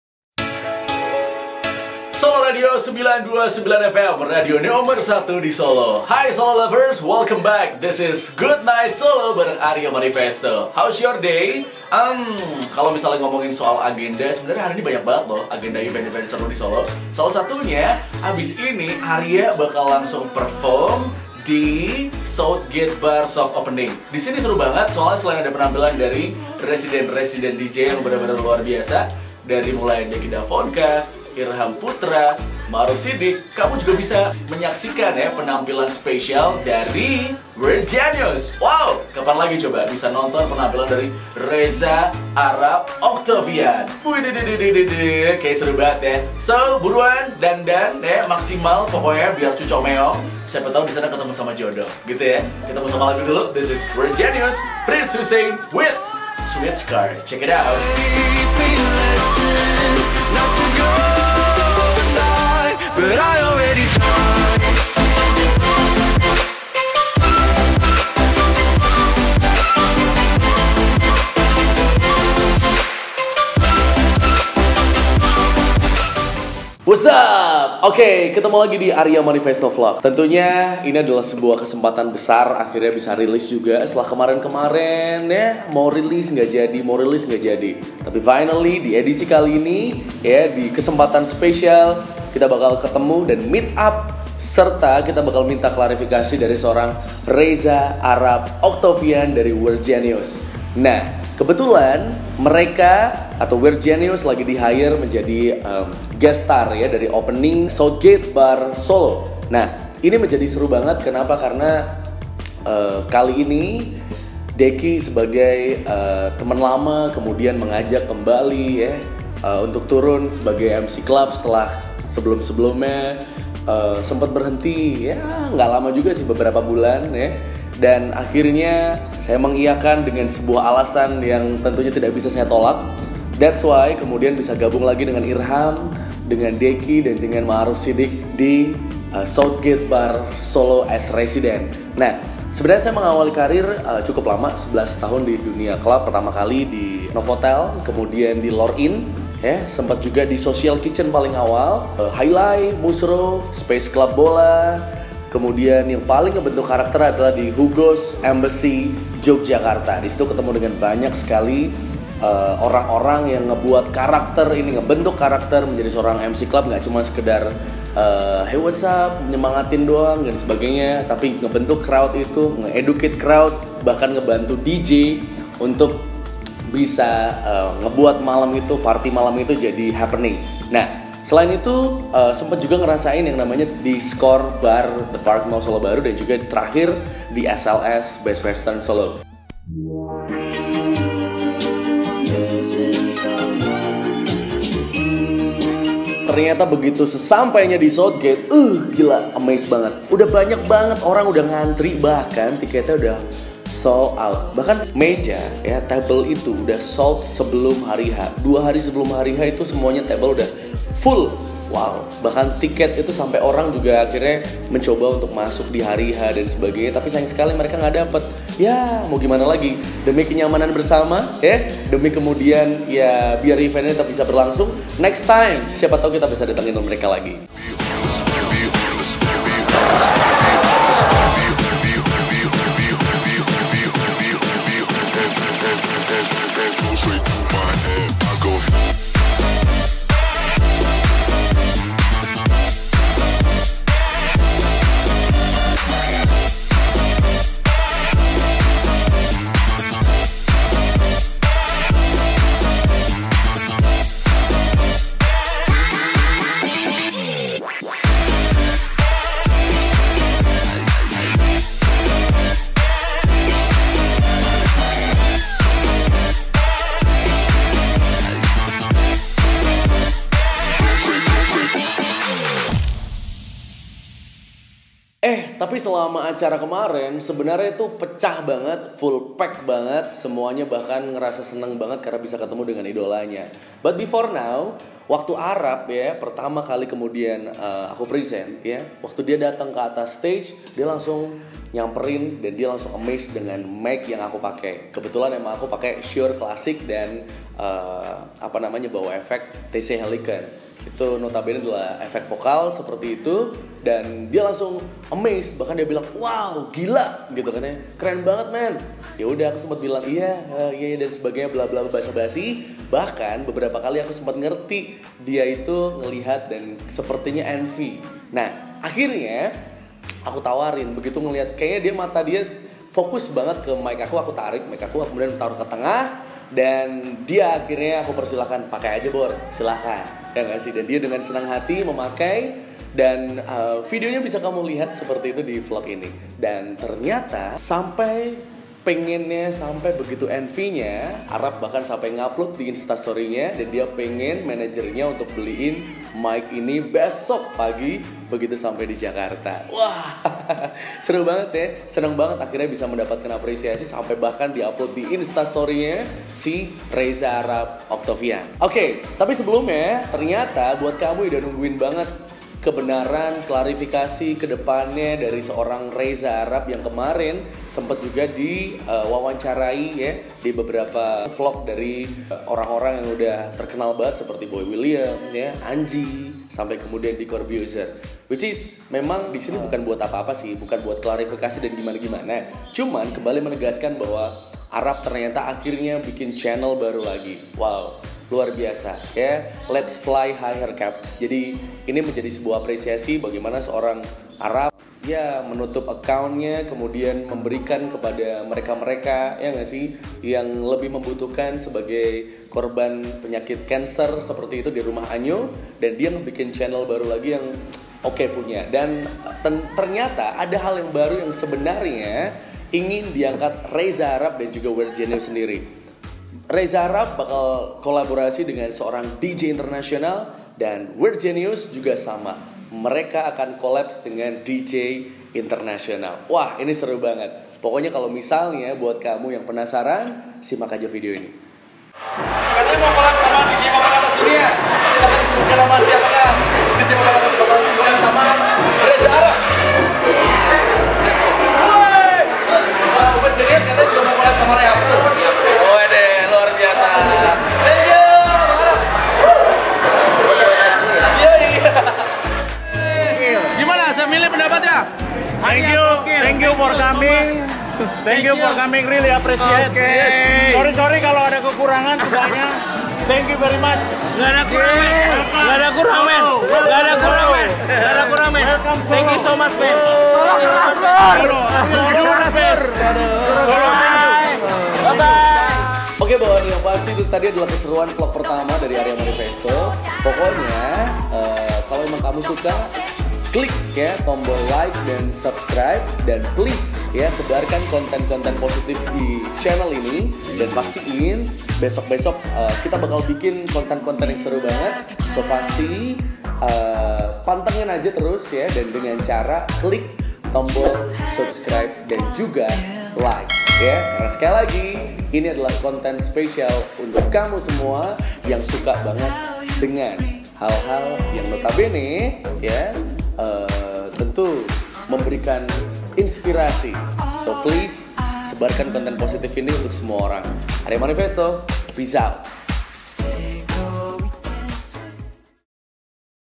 BIKIN ENVY REZA ARAP
Hai Bor !!
Yeah seru banget soft opening dari SOUTHGATE BAR Solo. Buat yang pernah dateng ke Social Kitchen sekarang tempat ini udah ganti nama dan konsep. Lebih gokilnya, di edisi pembukaan datengin WEIRD GENIUS. We O We. WOW !!

Pas banget sama beritanya sama Reza Arap yang sempet tutup channel dan kasih buat Rumah Anyo sama Netizen yang dibikin penasaran sama whats next from him ?

Ada satu hal yang bikin Reza Arap sempet iri sama aku disini. Makanya lo semua hukumnya wajib ain nonton video ini sampe kelar.

PEACE OUT YO